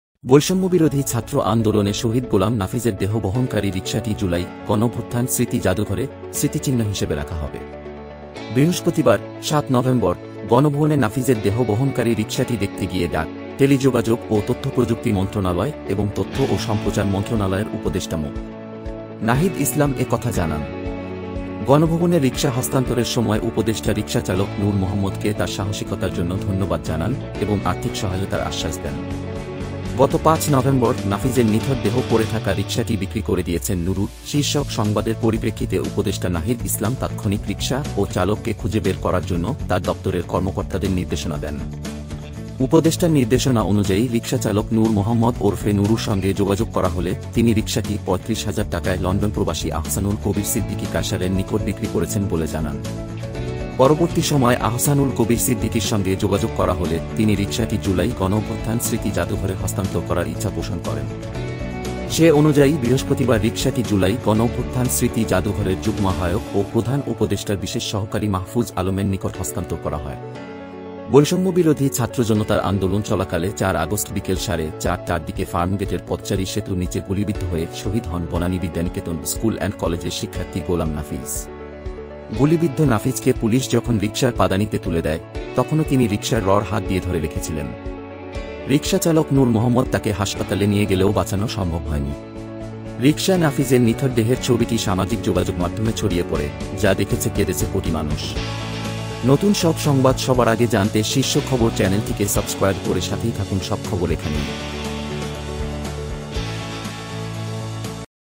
গণভবনে স্থান পাচ্ছে শহীদ নাফিজের নিথর দেহ বহন করা সেই রিকশা || শীর্ষ খবর
বৈষম্যবিরোধী ছাত্র আন্দোলনে শহীদ গোলাম নাফিজের দেহ বহনকারী রিকশাটি জুলাই গণঅভ্যুত্থান স্মৃতি জাদুঘরে স্মৃতিচিহ্ন হিসেবে রাখা হবে।
বৃহস্পতিবার (৭ নভেম্বর) গণভবনে নাফিজের দেহ বহনকারী রিকশাটি দেখতে গিয়ে ডাক, টেলিযোগাযোগ ও তথ্যপ্রযুক্তি মন্ত্রণালয় এবং তথ্য ও সম্প্রচার মন্ত্রণালয়ের উপদেষ্টা মো. নাহিদ ইসলাম এ কথা জানান।
গণভবনে রিকশা হস্তান্তরের সময় উপদেষ্টা রিকশাচালক নূর মোহাম্মদকে তার সাহসিকতার জন্য ধন্যবাদ জানান এবং আর্থিক সহায়তার আশ্বাস দেন।
গত ৫ নভেম্বর ‘নাফিজের নিথর দেহ পড়ে থাকা রিকশাটি বিক্রি করে দিয়েছেন নূরু’ শীর্ষক সংবাদের পরিপ্রেক্ষিতে উপদেষ্টা নাহিদ ইসলাম তাৎক্ষণিক রিকশা ও চালককে খুঁজে বের করার জন্য তার দপ্তরের কর্মকর্তাদের নির্দেশনা দেন।
উপদেষ্টার নির্দেশনা অনুযায়ী রিকশাচালক নূর মোহাম্মদ ওরফে নূরুর সঙ্গে যোগাযোগ করা হলে তিনি রিকশাটি পয়ত্রিশ হাজার টাকায় লন্ডনপ্রবাসী আহসানুল কবীর সিদ্দিকী কায়সারের নিকট বিক্রি করেছেন বলে জানান। পরবর্তী সময় আহসানুল কবীর সিদ্দিকীর সঙ্গে যোগাযোগ করা হলে তিনি রিকশাটি জুলাই গণঅভ্যুত্থান স্মৃতি জাদুঘরে হস্তান্তর করার ইচ্ছা পোষণ করেন।
সে অনুযায়ী বৃহস্পতিবার রিকশাটি জুলাই গণঅভ্যুত্থান স্মৃতি জাদুঘরের যুগ্ম-আহ্বায়ক ও প্রধান উপদেষ্টার বিশেষ সহকারী মাহফুজ আলমের নিকট হস্তান্তর করা হয়।

বৈষম্যবিরোধী ছাত্র-জনতার আন্দোলন চলাকালে ৪ আগস্ট বিকেল সাড়ে ৪টার দিকে ফার্মগেটের পদচারী সেতুর নিচে গুলিবিদ্ধ হয়ে শহীদ হন বনানী বিদ্যানিকেতন স্কুল অ্যান্ড কলেজের শিক্ষার্থী গোলাম নাফিজ।
গুলিবিদ্ধ নাফিজকে পুলিশ যখন রিকশার পাদানিতে তুলে দেয়, তখনো তিনি রিকশার রড হাত দিয়ে ধরে রেখেছিলেন। রিকশাচালক নূর মোহাম্মদ তাকে হাসপাতালে নিয়ে গেলেও বাঁচানো সম্ভব হয়নি। রিকশায় নাফিজের নিথর দেহের ছবিটি সামাজিক যোগাযোগমাধ্যমে ছড়িয়ে পড়ে, যা দেখেছে কেঁদেছে কোটি মানুষ।